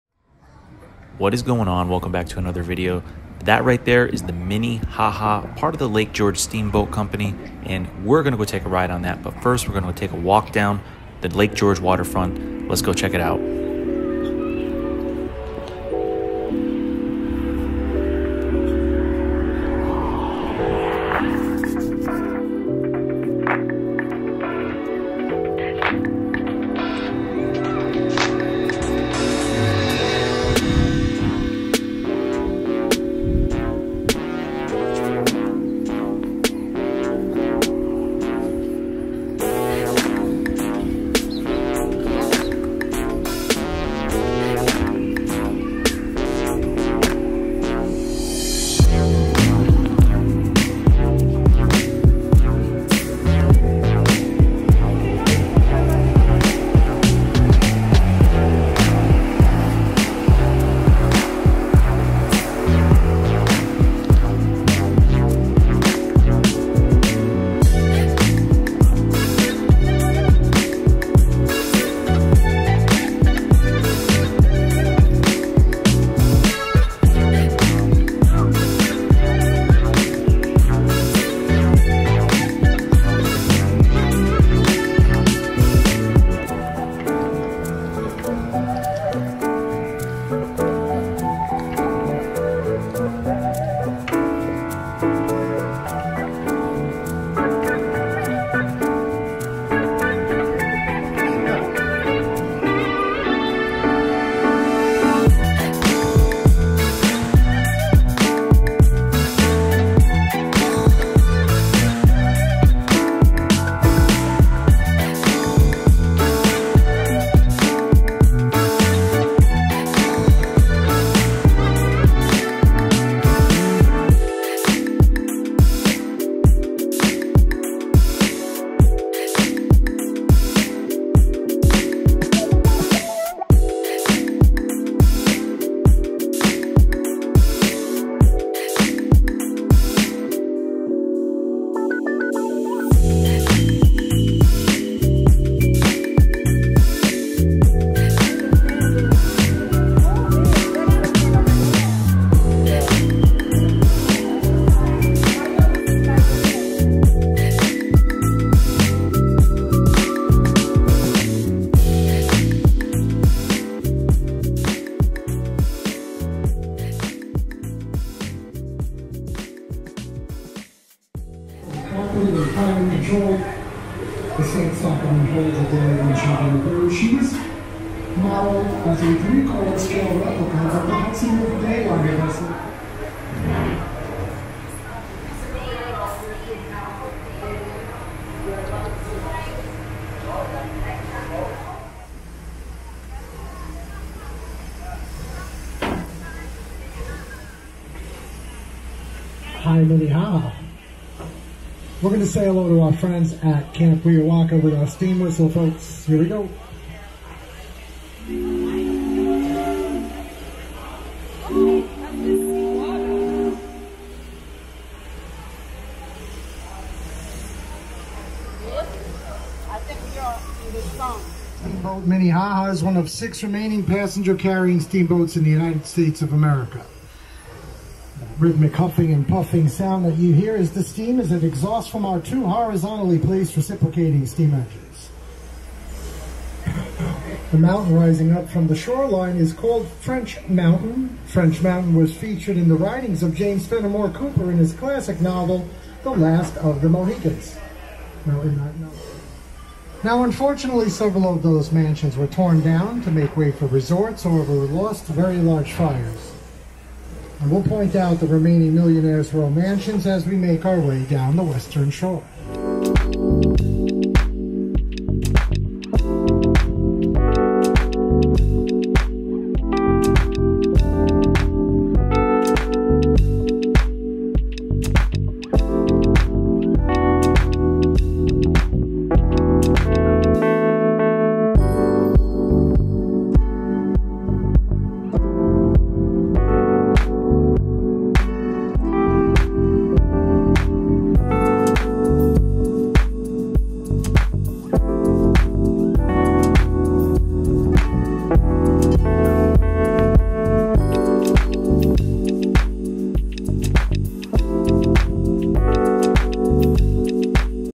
Lake George Steamboat Company | The Minne Ha Ha
Robert Fulton established the Lake George Steamboat Company, America's first commercially successful steamboat service. His steam-powered paddle boat, the Clermont, sailed up the Hudson River from New York City to Albany, arriving on August 19, 1807, at the state capitol. The first steamboats arrived on Lake George in 1817. The settlements along the lake's shores were small, local business was limited, and there was a general aversion to steamboats as being somehow associated with the Devil. The success of steam on nearby Lake Champlain, however, was undeniable and likely contagious. The New York State Legislature incorporated a company to operate commercial shipping on Lake George on April 15, 1817.

The Minne-Ha-Ha is one of America's last steam paddle wheel ships. She is ideal for children and families with busy schedules. She goes on an hour-long cruise six times a day, plus a Friday night Pirate Adventure Cruise. In between cruises, guests will be entertained by the Minne's authentic steam calliope concert. The Minne also hosts a new live entertainment event called "Rock the Dock." It doesn't get much better than sailing along to good music in the cool summer evening air!